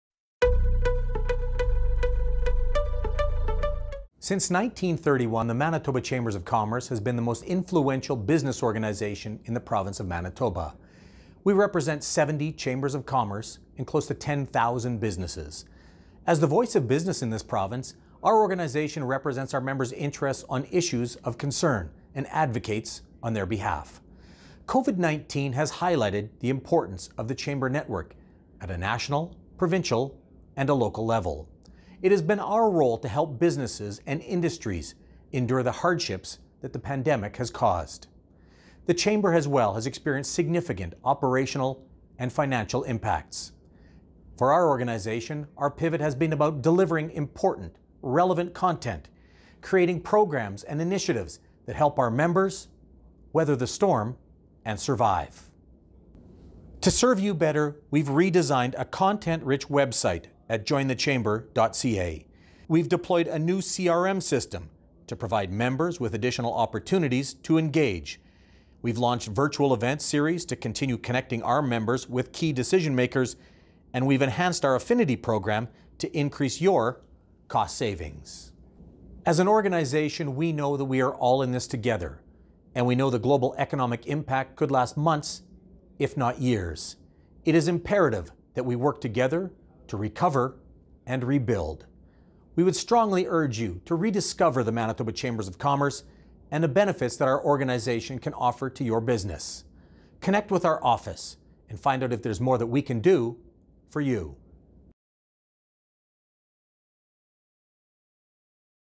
The Voice of Business - How We've Pivoted to Serve You Better
In our first video of this new series, MB Chambers President & CEO, Chuck Davidson, discusses the ways we have pivoted our focus to best serve our members and the broader Manitoba business community amid COVID-19.

Visit jointhechamber.ca today to learn more!